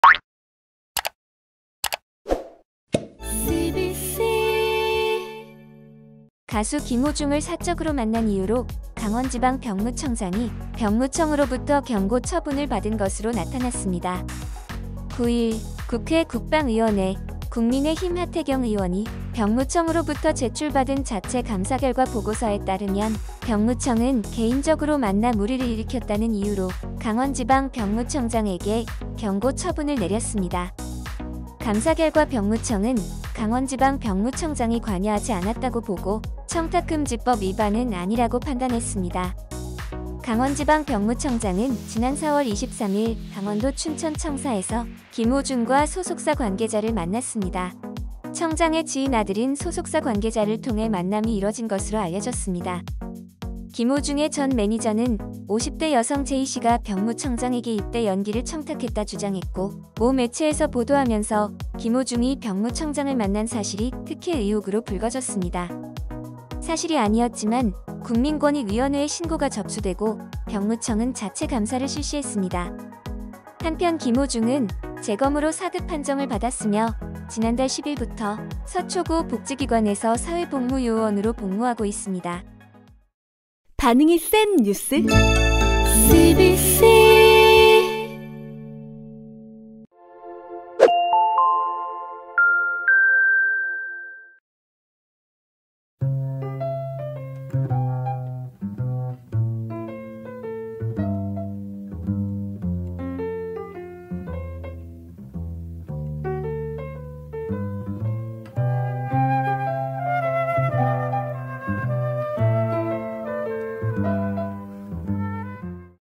김호중 병역 청탁 의혹…병무청 자체감사결과 관여없음 결론 | CBCNEWS, CBC뉴스
'트바로티' 김호중의 인기비결은?
지금 팬들의 영상을 확인하세요!!

[CBC뉴스] 가수 김호중을 사적으로 만난 이유로 강원지방병무청장이 병무청으로부터 '경고' 처분을 받은 것으로 나타났다.

9일 국회 국방위원회 국민의힘 하태경 의원이 병무청으로부터 제출받은 자체 감사결과 보고서에 따르면 병무청은 개인적으로 만나 물의를 일으켰다는 이유로 강원지방병무청장에게 경고 처분을 내렸다. 감사결과 병무청은 강원지방병무청장이 관여하지 않았다고 보고 청탁금지법 위반은 아니라고 판단했다.

앞서 강원지방병무청장은 지난 4월 23일 강원도 춘천 청사에서 김호중과 소속사 관계자를 만났다. 청장의 지인 아들인 소속사 관계자를 통해 만남이 이뤄진 것으로 알려졌다.

김호중의 전 매니저는 50대 여성 J씨가 병무청장에게 입대 연기를 청탁했다 주장했고 모 매체에서 보도하면서 김호중이 병무청장을 만난 사실이 특혜 의혹으로 불거졌다. 사실이 아니었지만 국민권익위원회에 신고가 접수되고 병무청은 자체 감사를 실시했다.

한편 김호중은 재검으로 4급 판정을 받았으며 지난달 10일부터 서초구 복지기관에서 사회복무요원으로 복무하고 있다.